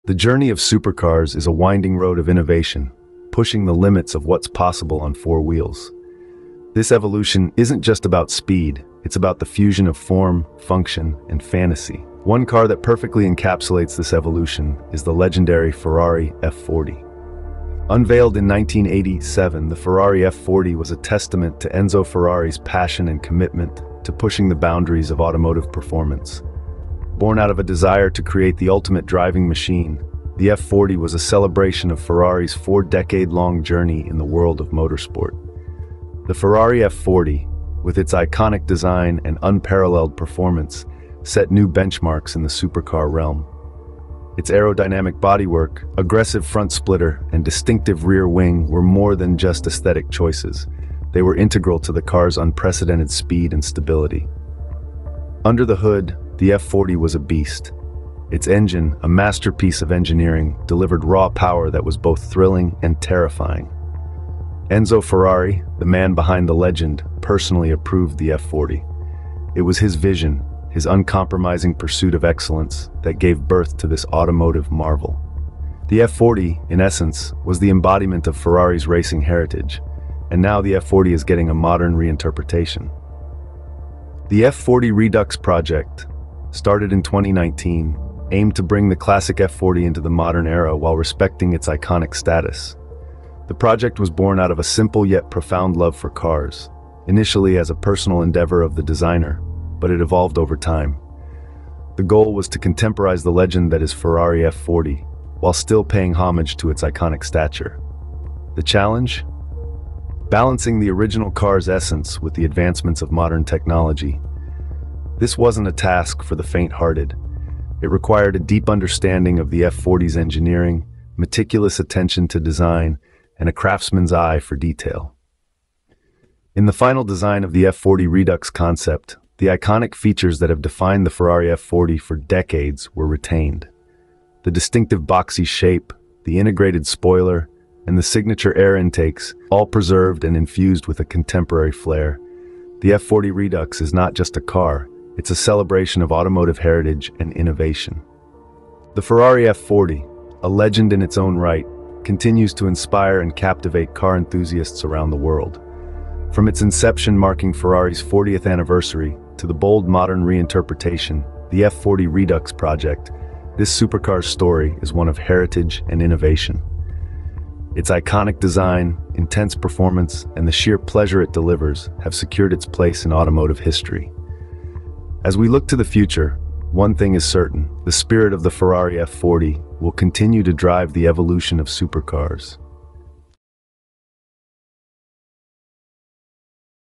The Icon Reborn - NEW 2025 Ferrari F40
The Ferrari F40 is a car that has earned its reputation as a "beast of a supercar," and it has solidified its place in automotive history. Its iconic design, unparalleled performance, and sheer driving pleasure continue to capture the hearts of car enthusiasts around the world. First introduced in 1987 to celebrate Ferrari's 40th anniversary, the F40 was designed to be the ultimate road-going Ferrari, embodying the brand's racing heritage and pushing the boundaries of performance. The F40 was the last car to be personally approved by Enzo Ferrari himself, and it was intended to be a no-compromise, stripped-down, and focused driving machine.The F40's design is instantly recognizable, with its low-slung, aerodynamic bodywork, aggressive front splitter, and iconic rear wing. The Ferrari F40 is a legendary car, and over the years, there have been many attempts to reinterpret its design. However, the real challenge lies in creating a design that pays homage to the original while asserting its unique identity. This requires a delicate balance between respecting the classic lines of the F40 and infusing them with a modern and contemporary flair.
Designed by Naoto Kobayashi and collaboration with Alejandro Miranda Rosaslanda.
The F40 Redux project is a celebration of automotive heritage and innovation. It pays homage to the legendary Ferrari F40 while embracing modernity and pushing the boundaries of design. The F40 Redux project began in 2019, driven by a strong passion for cars. It was originally a personal project of the designer but evolved. The goal was to update the legendary Ferrari F40 for today's car enthusiasts while still respecting its iconic status.A major challenge of the F40 Redux project was to balance the original car's essence with modern technology. This needed a good understanding of the F40's engineering and design, along with attention to detail and craftsmanship.In the final design of the F40 Redux concept, the designers retained the iconic features that have defined the Ferrari F40 for decades. These features include the distinctive boxy shape, integrated spoiler, and signature air intakes. These elements, which have become synonymous with the F40, were preserved to ensure a seamless transition from the past to the present.